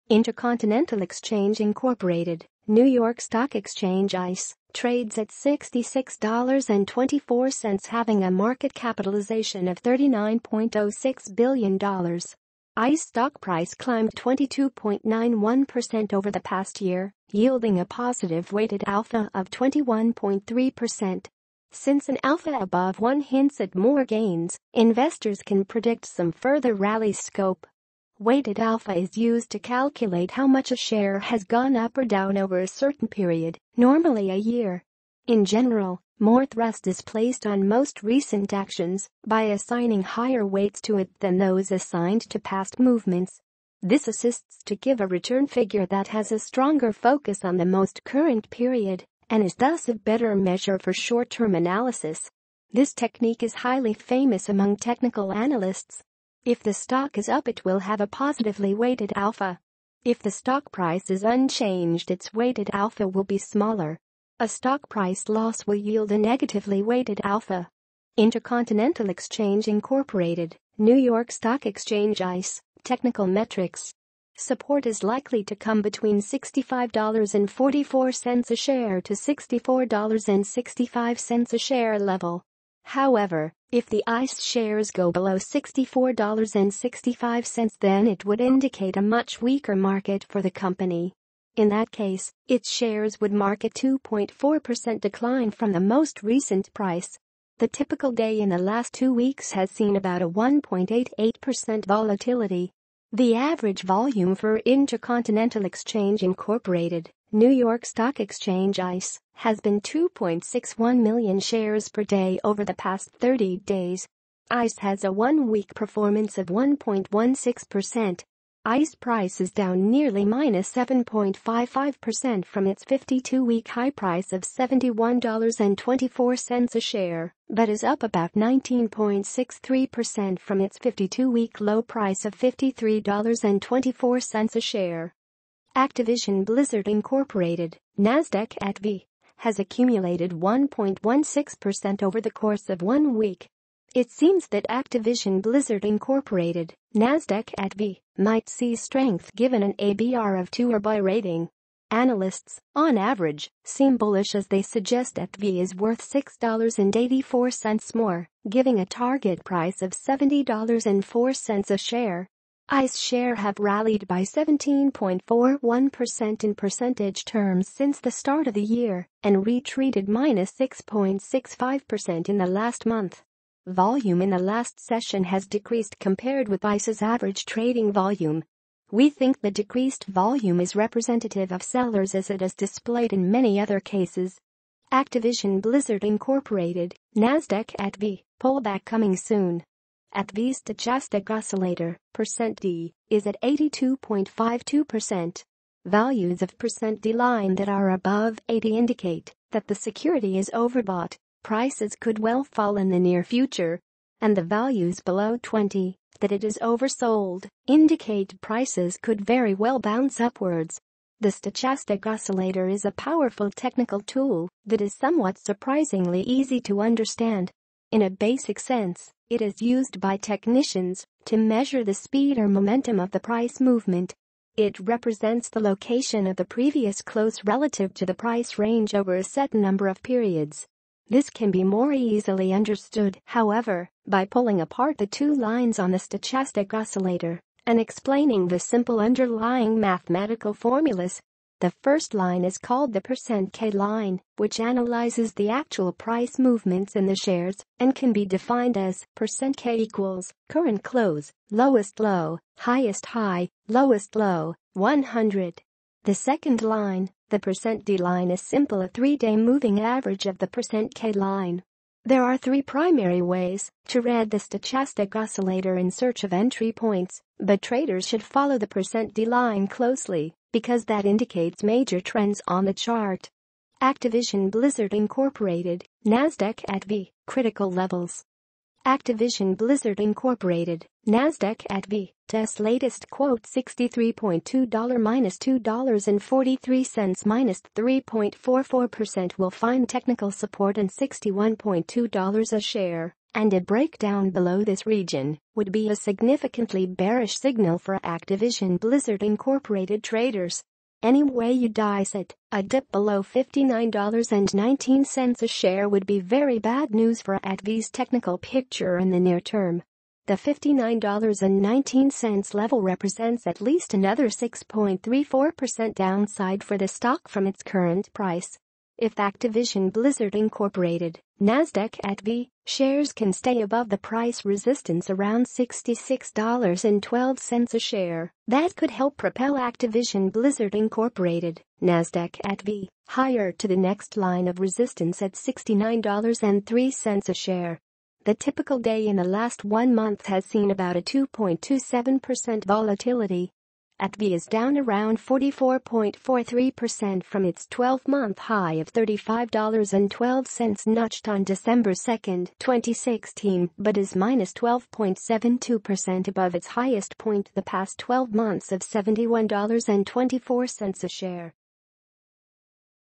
Let’s Put These Technical Indicators To Work: Intercontinental Exchange, Inc. (ICE),
Intercontinental Exchange, Inc. (NYSE:ICE) trades at $66.24 having a market capitalization of $39.06 billion. ICE stock price climbed 22.91% over the past year, yielding a positive weighted alpha of 21.3 percent. Since an alpha above 1 hints at more gains, investors can predict some further rally scope.